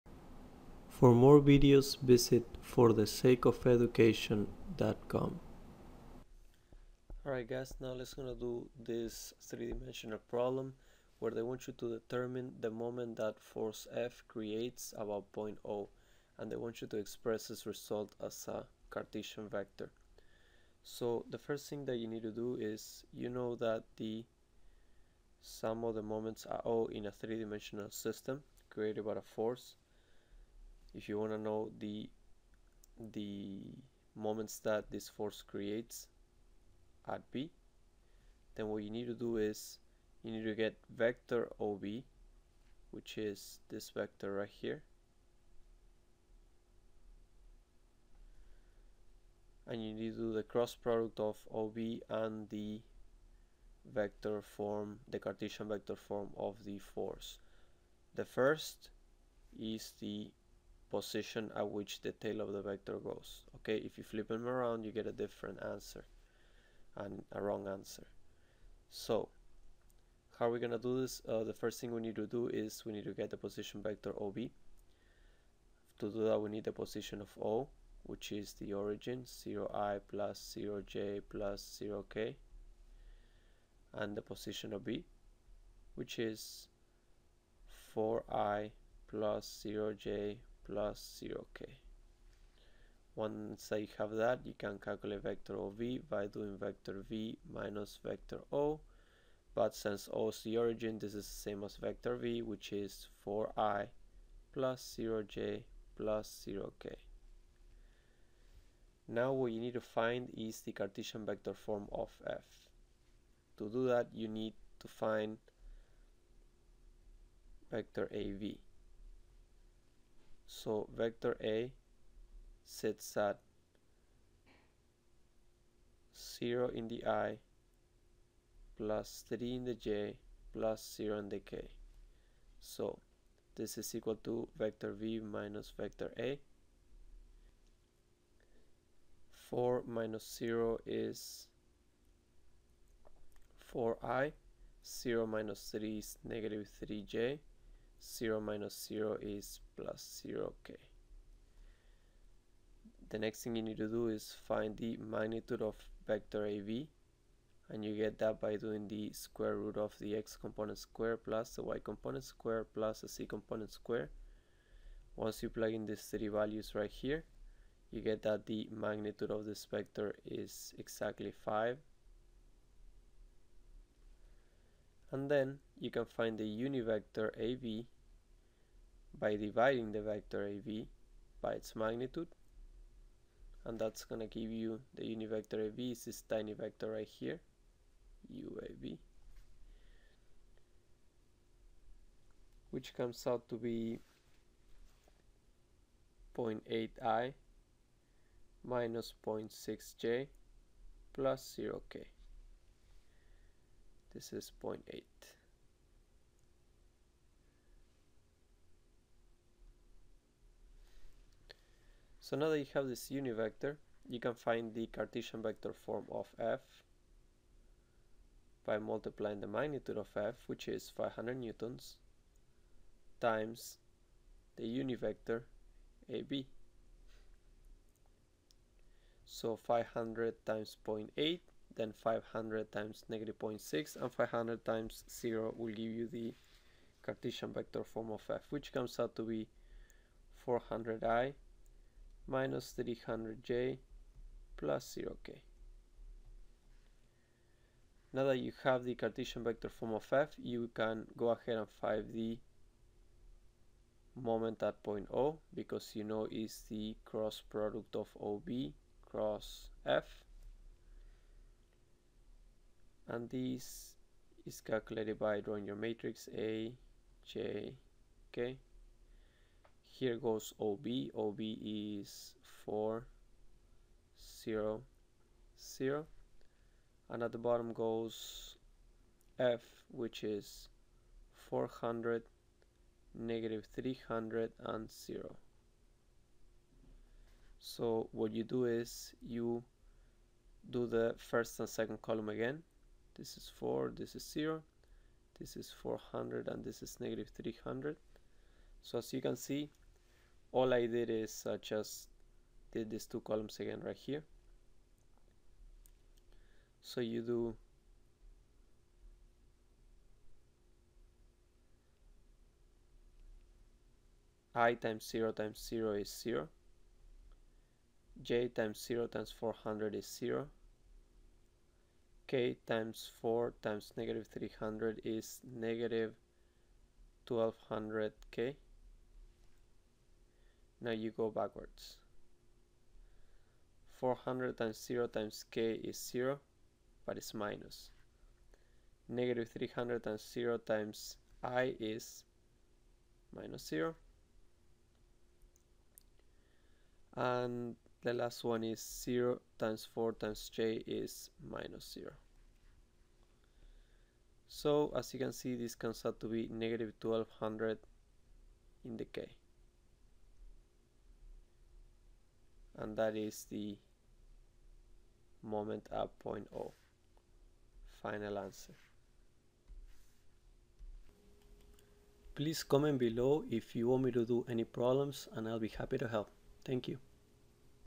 Determine the moment of force F about point O
Express the result as a Cartesian vector.